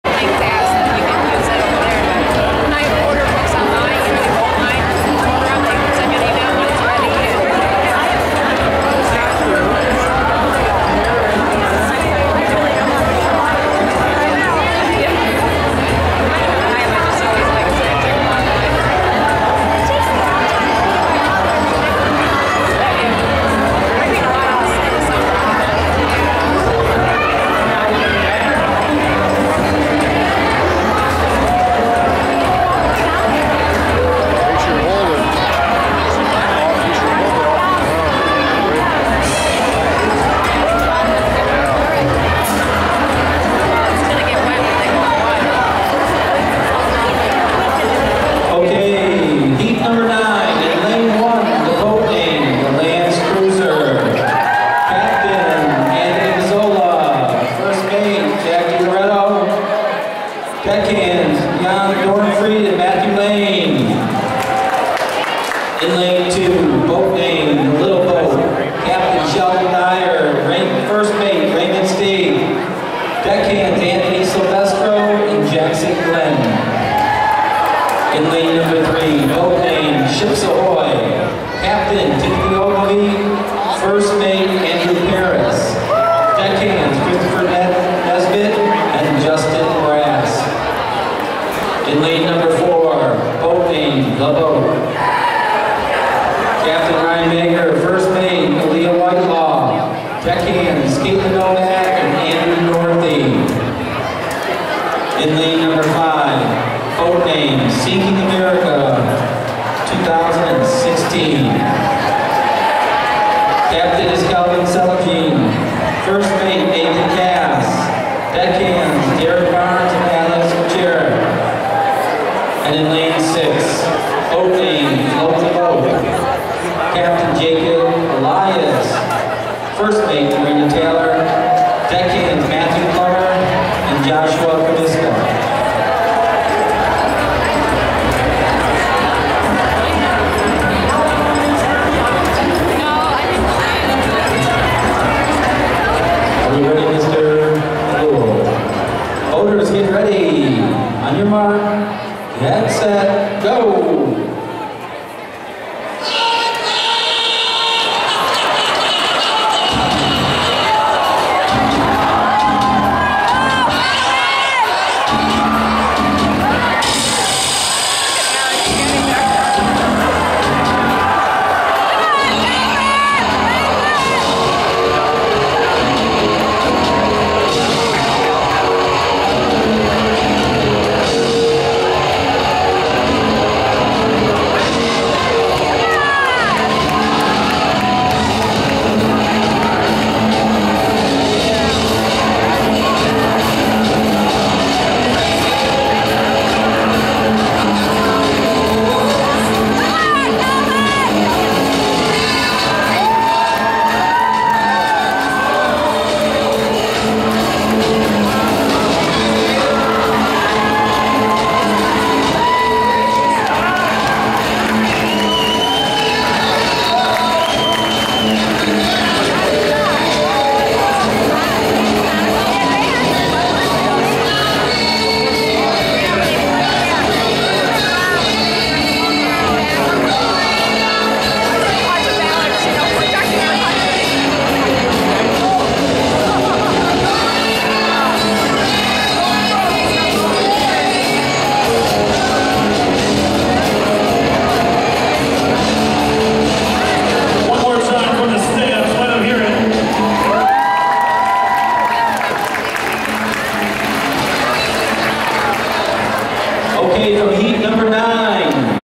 Cardboard boat race heat 9
L'Anse Creuse High School cardboard boat regatta heat 9.